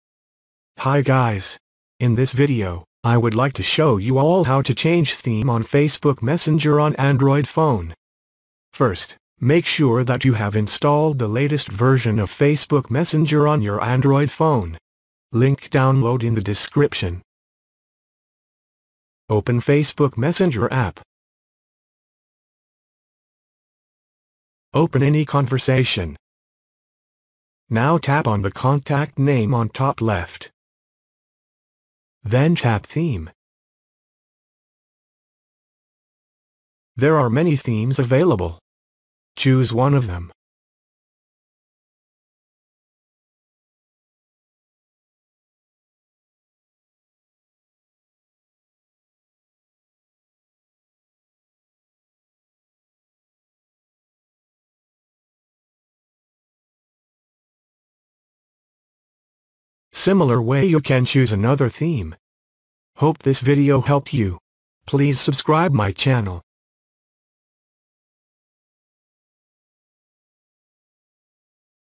How to Change Theme on Facebook Messenger on Android phone
hi guys, in this video, i would like to show you all how to change theme on facebook messenger on Android phone.

first, make sure that you have installed the latest version of Facebook messenger on your Android phone,